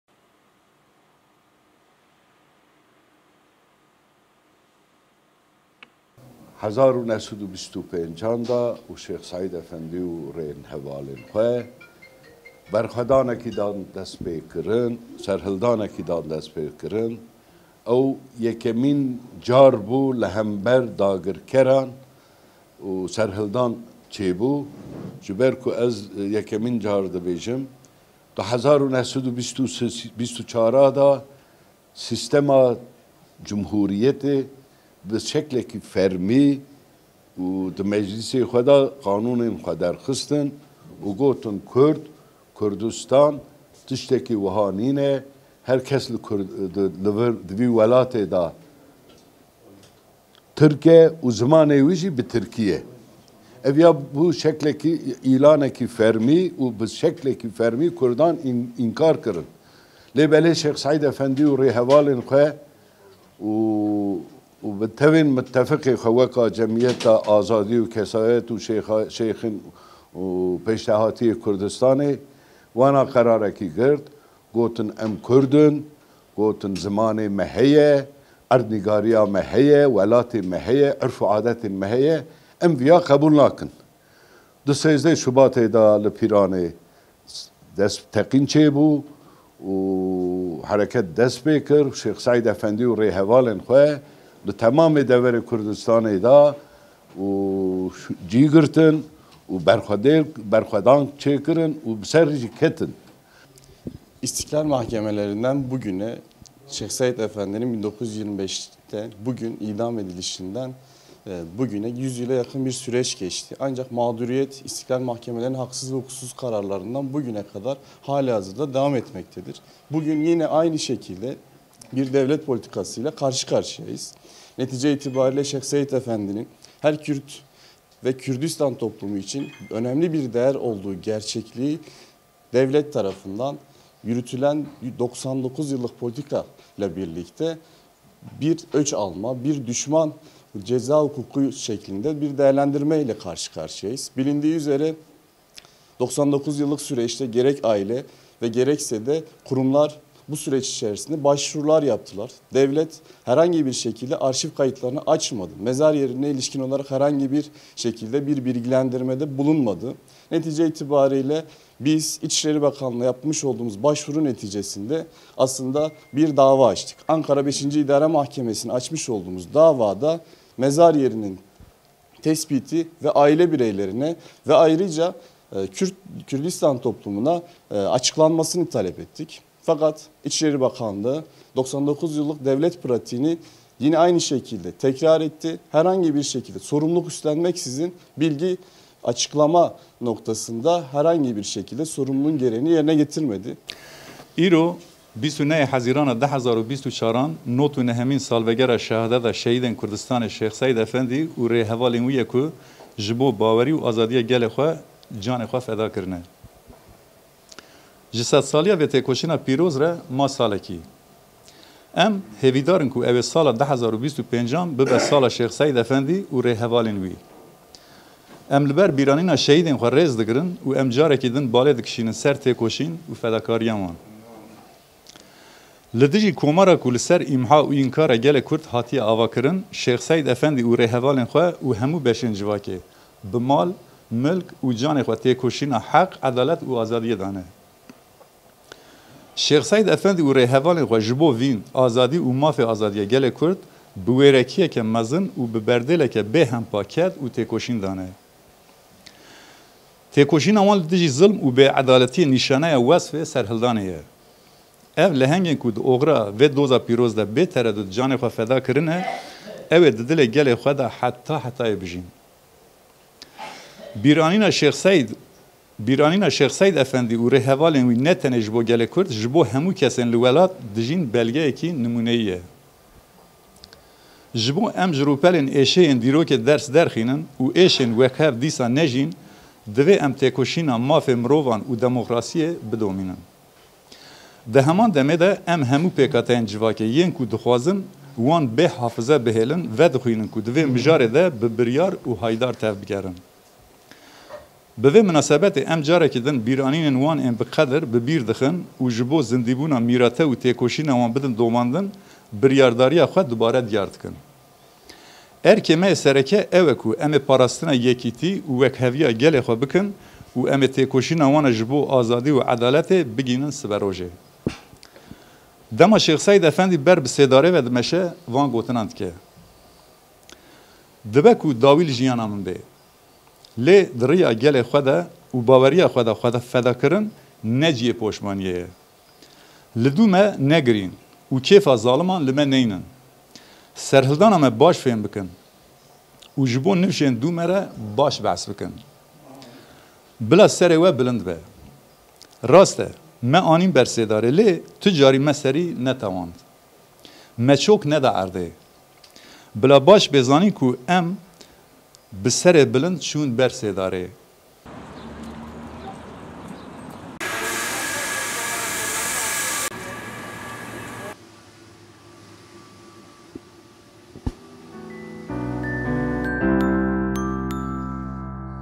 ŞÊX SEÎD Û 46 HEVALÊN XWE LI CIHÊ DARVEKIRINÊ HATIN BIBÎRANÎN: BILA CIHÊ GORÊN WAN BÊN EŞKEREKIRIN
Şêx Seîd û hevalên xwe yên dozê di salvegera 99’emîn de li cihê lê hatibûn darvekirin, hatin bibîranîn. Di daxuyaniyê de hat destnîşankirin ku ji bo gora Şêx Seîd û gorên hevalên wî bên dîtin dê têkoşîn berdewam bike.